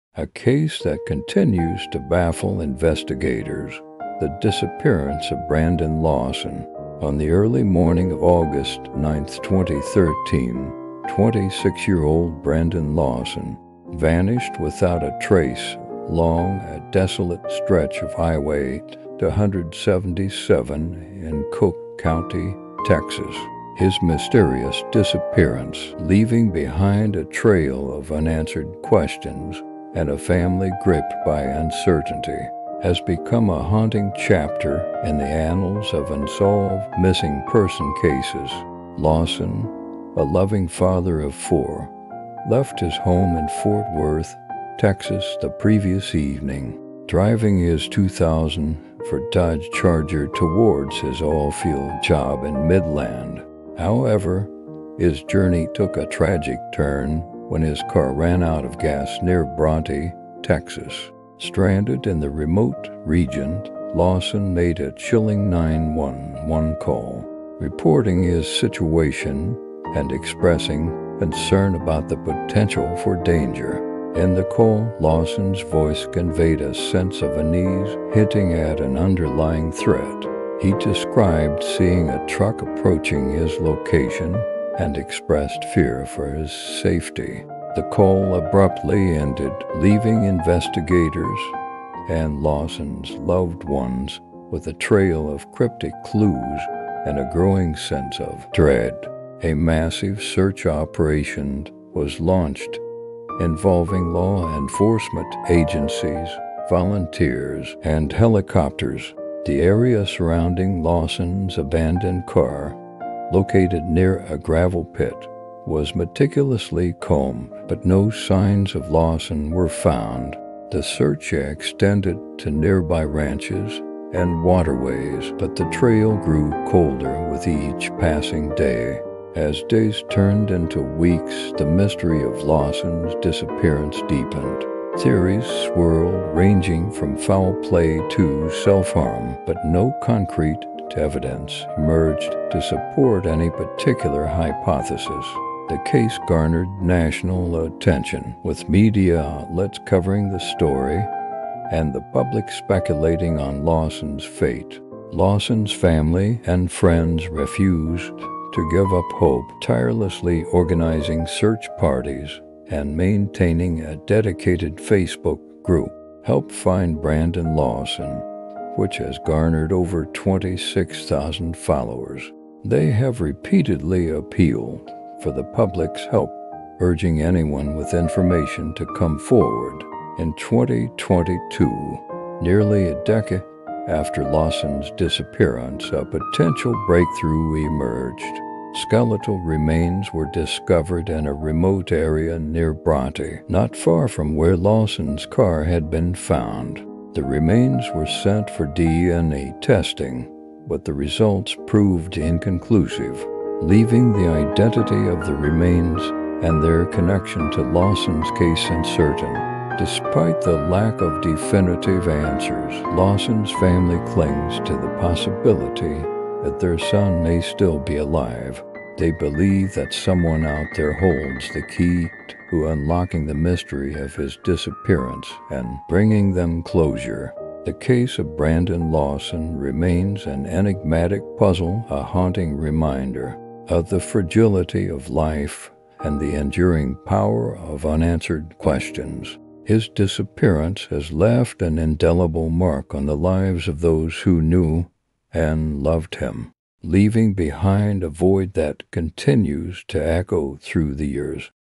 A Case That Continues to Baffle Investigators - The Disappearance of Brandon Lawson
On the early morning of August 9, 2013, 26-year-old Brandon Lawson vanished without a trace along a desolate stretch of Highway 277 in Coke County, Texas. His mysterious disappearance, leaving behind a trail of unanswered questions and a family gripped by uncertainty, has become a haunting chapter in the annals of unsolved missing person cases. @Myzteries1